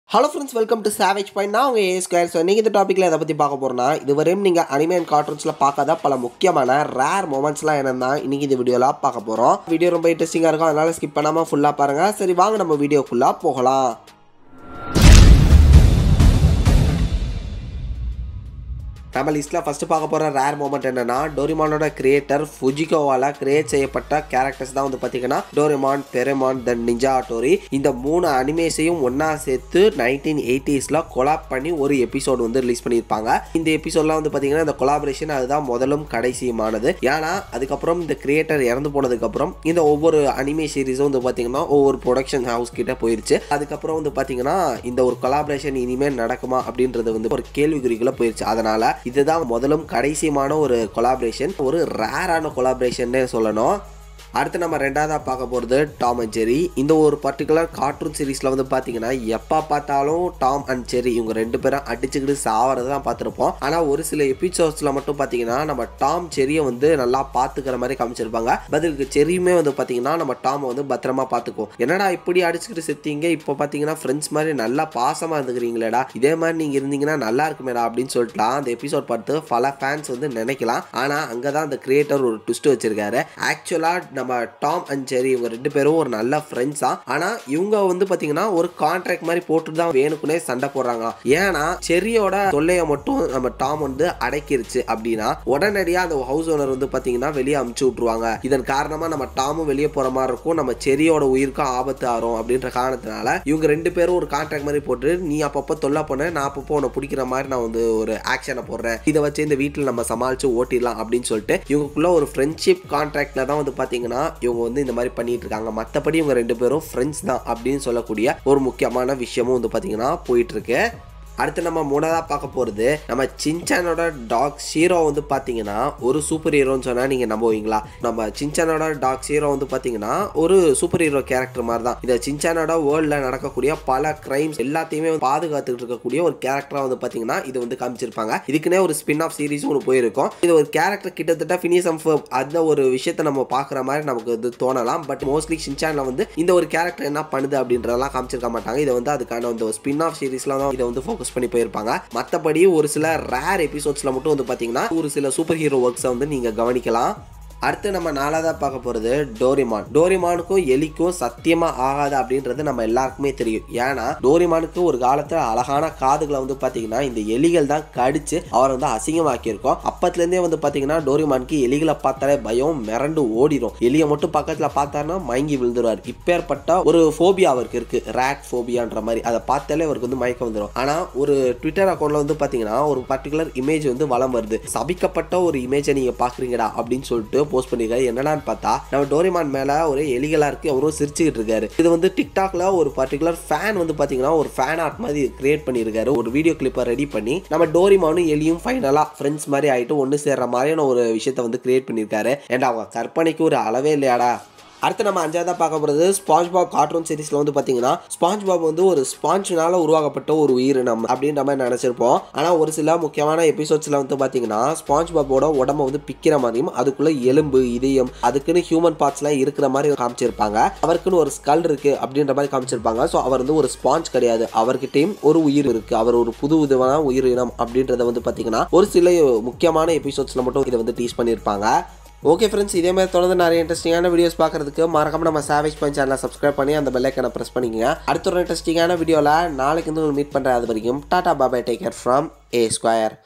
Most Rarest Moments & Interesting Facts in Cartoons & Animes | Savage Point
Hello friends
Welcome to Savage Point.
In this video we are going to see Most Rarest Moments & Interesting Facts in Cartoons & Animes.